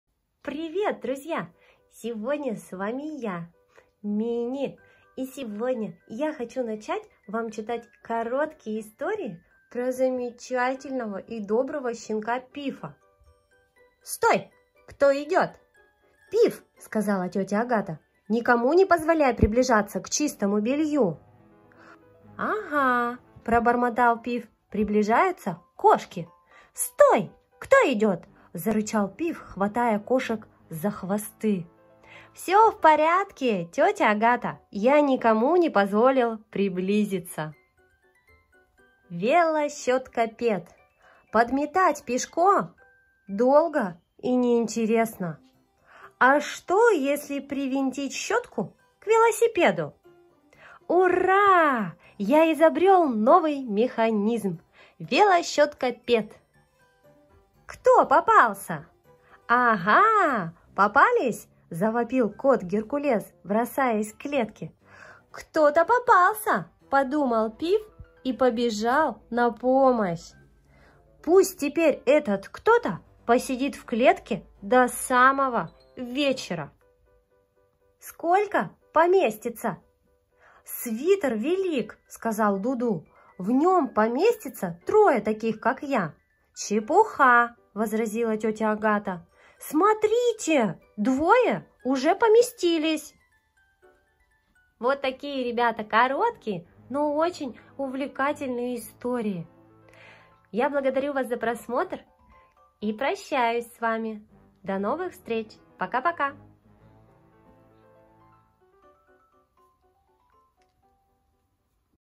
Детские истории «Приключения Пифа» - читаем вслух. Г. Остер
Знаете ли вы, ребята, что когда ваши папы и мамы были маленькие, они хохотали с утра до вечера, стоило им только увидеть картинки про замечательного французского пса по имени Пиф.
Теперь ваша очередь. Пора вам познакомиться с этим хитрым, честным, добрым и находчивым шалопаем. А заодно и со всей его шумной компанией.
В этой книге «Все приключения Пифа» собраны маленькие весёлые истории про знаменитого пёсика Пифа — большого проказника с добрым сердцем. Пиф, его верный друг — мальчик по имени Дуду, и его недруг чёрный кот Геркулес давно стали любимыми героями ребят в разных странах. Великолепные, остроумные иллюстрации к этим забавным историям сделал известный художник В. Сутеев.

Пересказ с французского.
Литературно-художественное издание Для детей до 3-х лет.
Один из рассказов:

Стой! кто идёт?

— Пиф, — сказала тётя Агата, — никому не позволяй приближаться к чистому белью.

— Ага! — пробормотал Пиф. — Приближаются кошки.

— Стой! Кто идёт? — зарычал Пиф, хватая кошек за хвосты.

— Всё в порядке, тётя Агата. Я никому не позволил приблизиться.